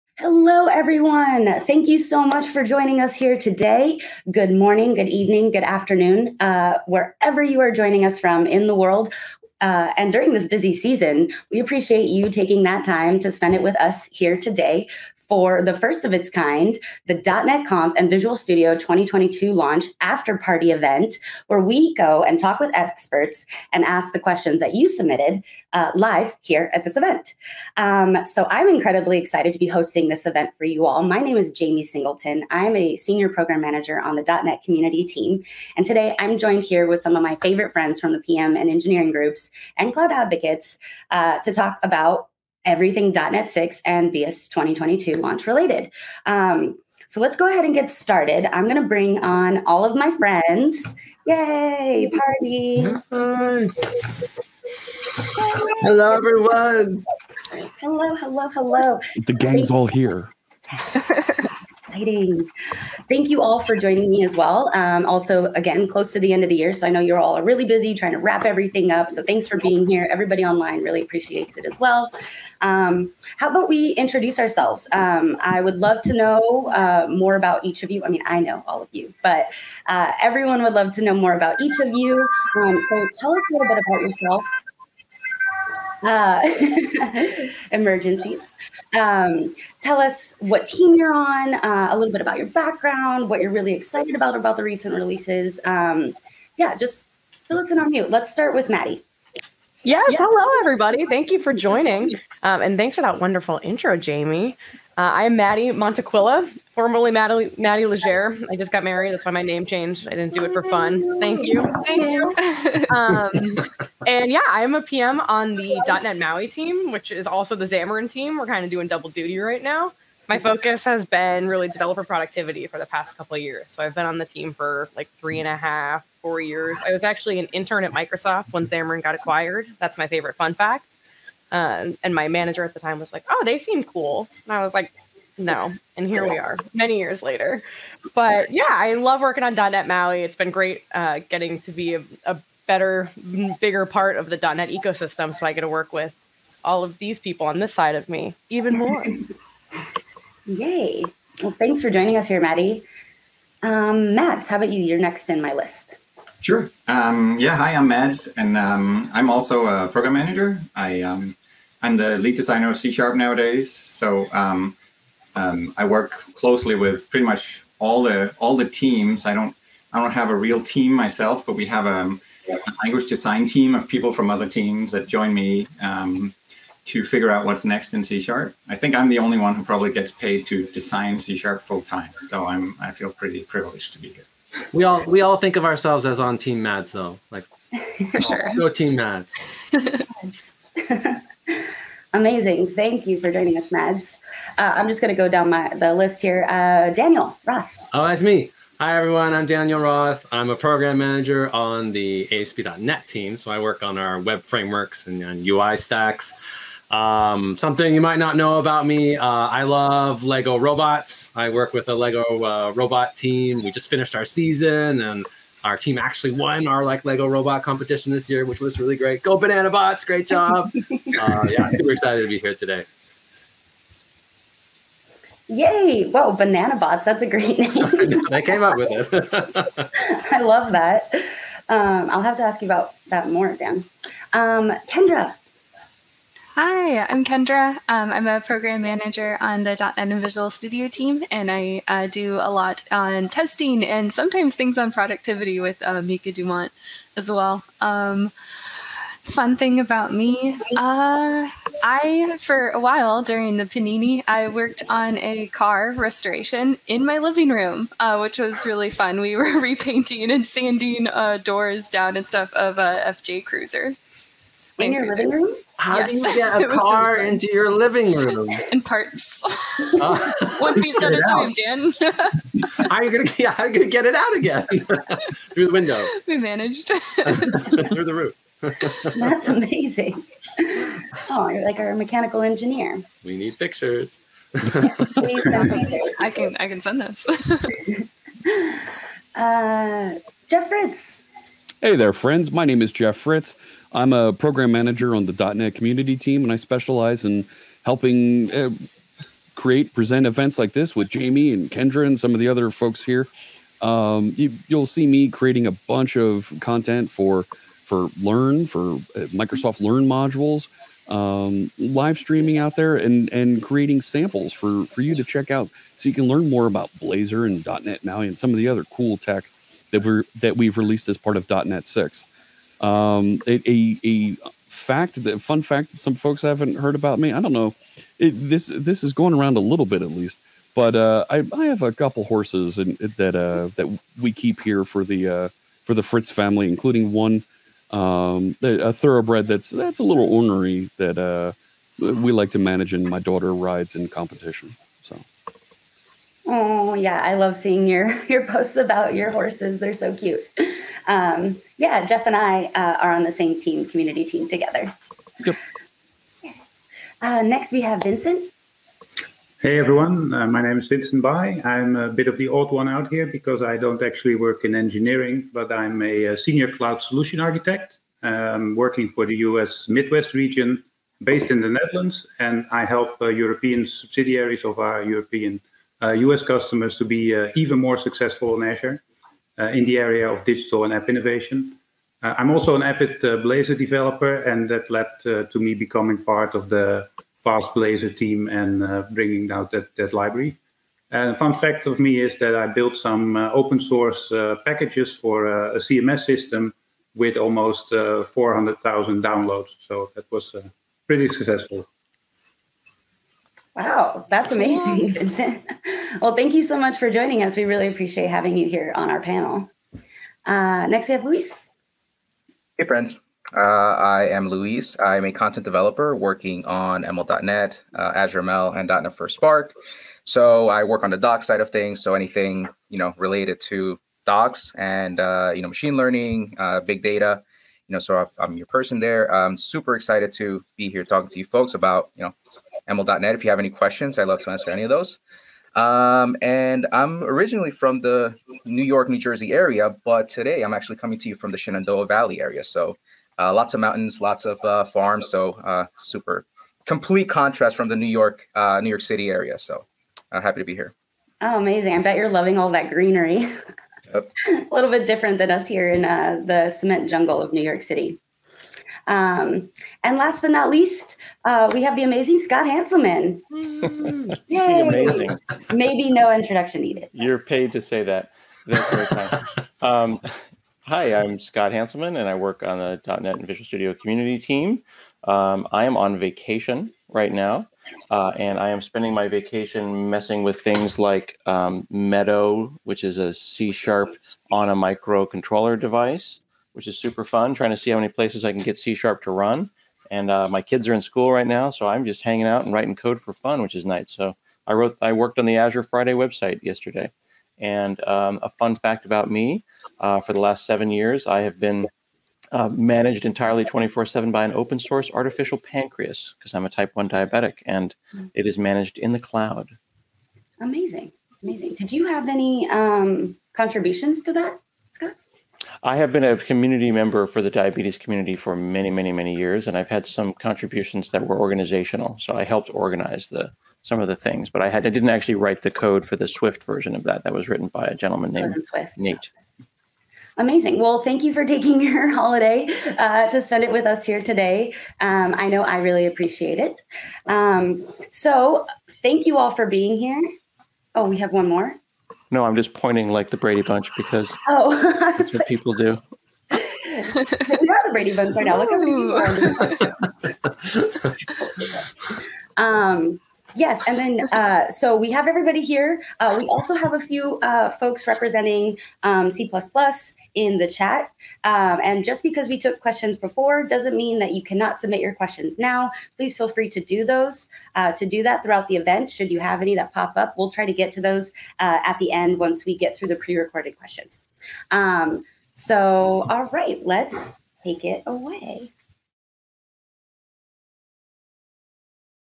Introductions & Meet the Team | .NET Conf & VS2022 After Party Q&A
Hear from .NET Conf and VS2022 speakers and experts who answer questions directly from the audience. Learn more about the recent launch of Visual Studio 2022 and .NET 6, including tips and tricks, new features with the latest releases, and connect with others in your community.

🏫 Learn C#, F#, and .NET with free self-guided learning from Microsoft Learn